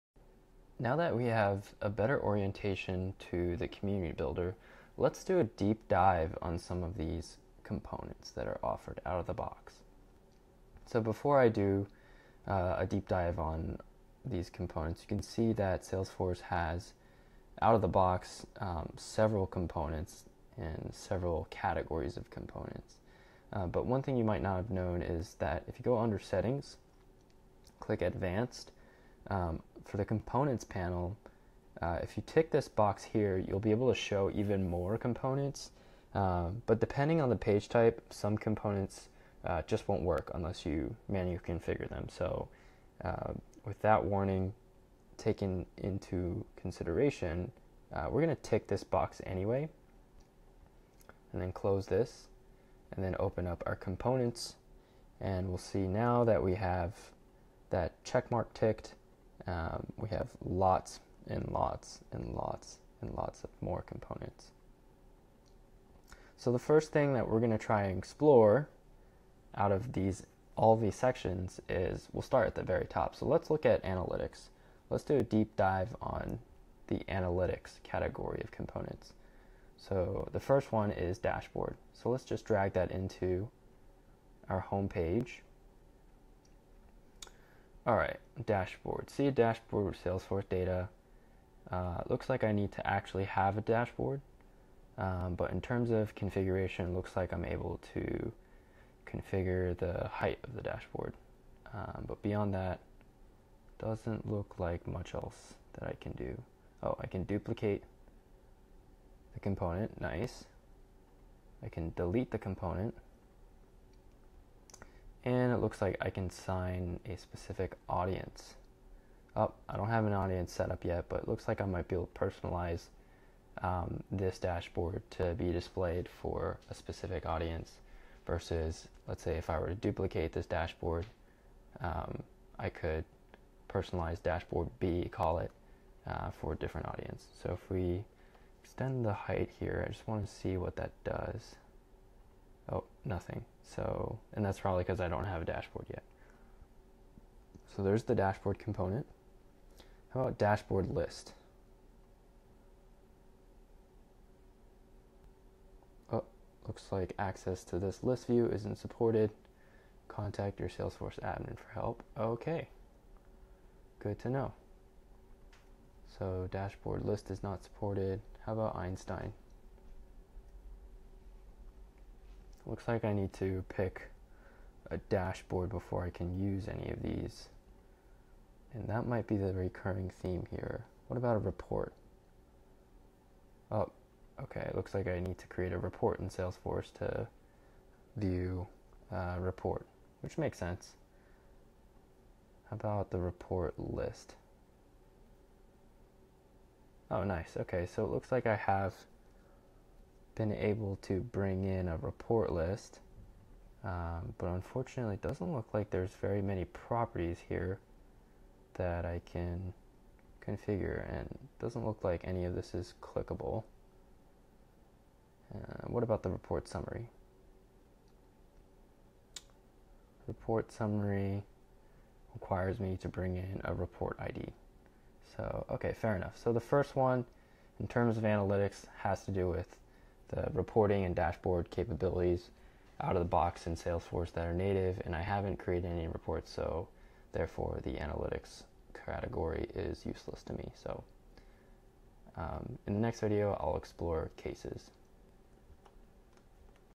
Deep-Dive: Lightning Components - Analytics
Watch me click through and explore Salesforce's Lightning Components, starting with their Analytics category.

This screen capture was created testing the September 2019 release of Salesforce.